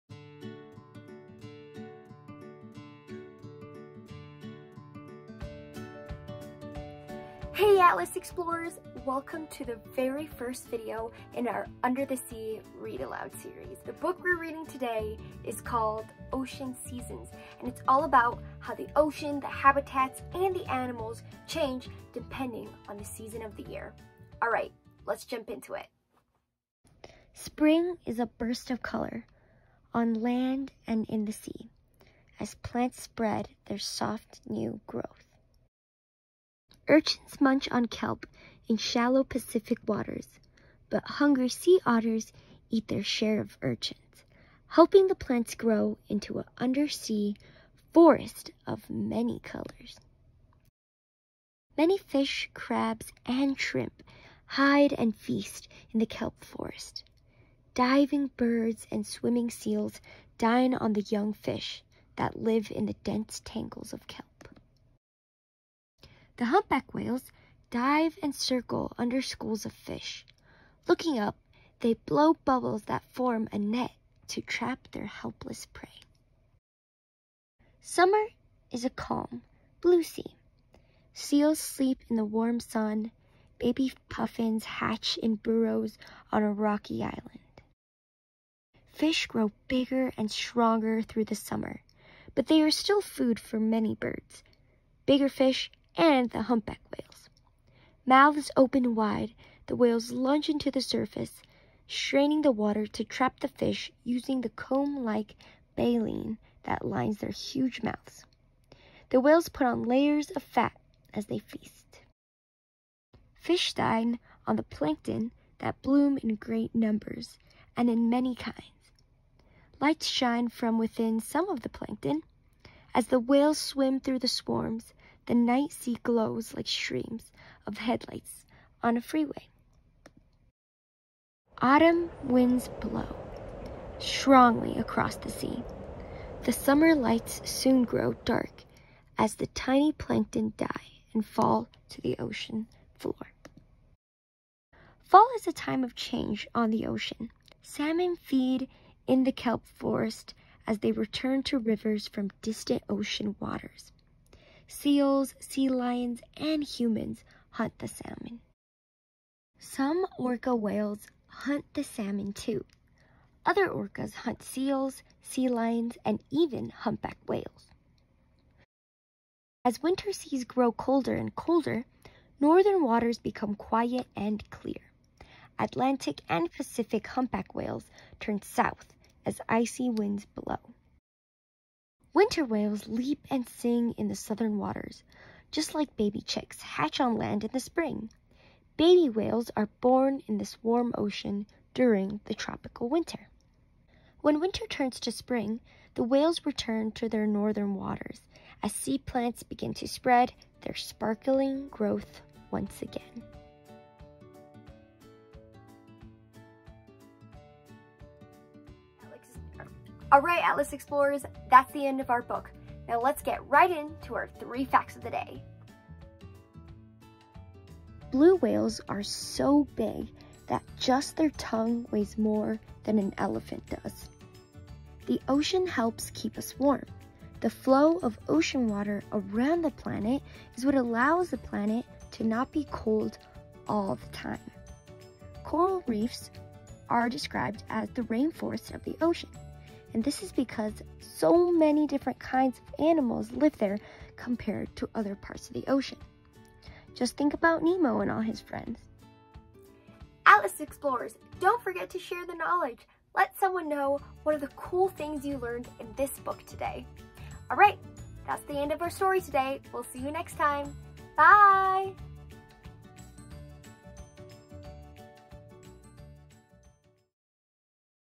Marine Biology for Kids!- "Ocean Seasons" Read Aloud (marine habitats and migration)
Hi Atlas Readers! Today we are reading the book “Ocean Seasons” This book is all about how ocean animals migrate and how underwater habitats change depending on the seasons! Head to our playlist tab and click on the  Under the Sea Read Aloud Series playlist to read more about the ocean with Brisa and Mr. Banana!

Book: What Can Live in the Ocean?
Author:  Ron Hirschi
Illustrator: Kirsten Carlson